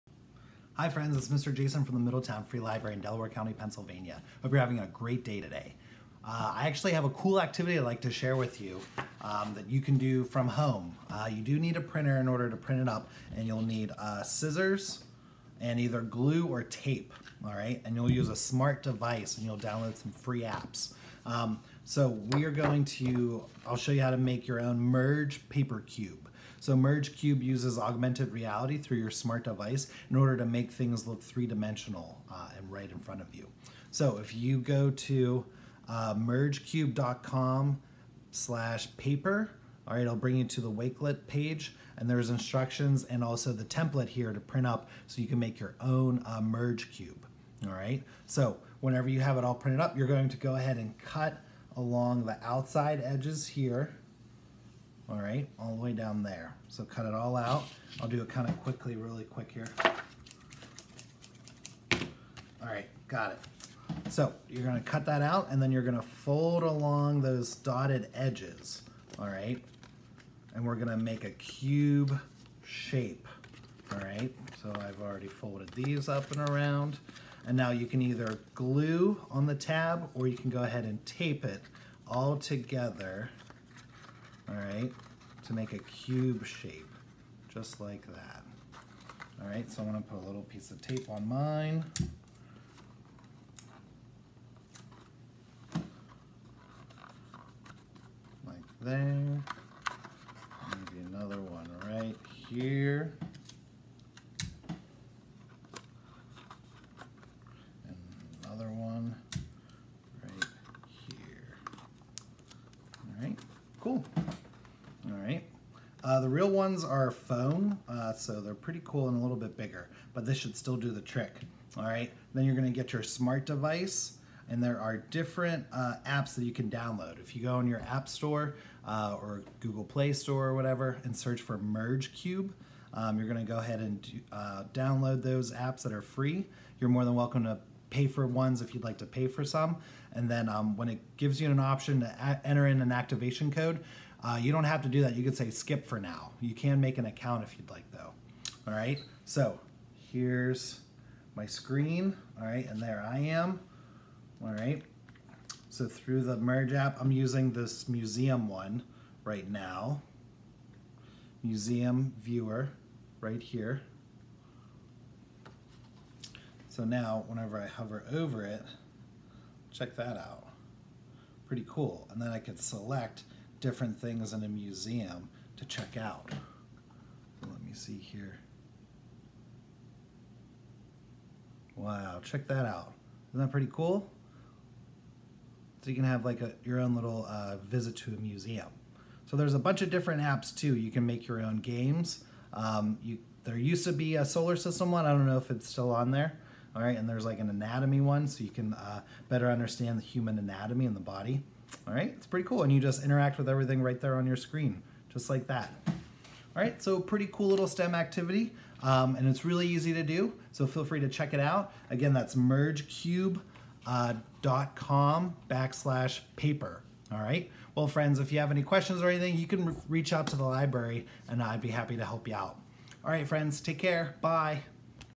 Merge Paper Cube w/ MFL
Mr. Jason from the Middletown Free Library shows you how to make & use your own paper Merge Cube!

Reach out to Mr. Jason at the library if you have any questions or need some help!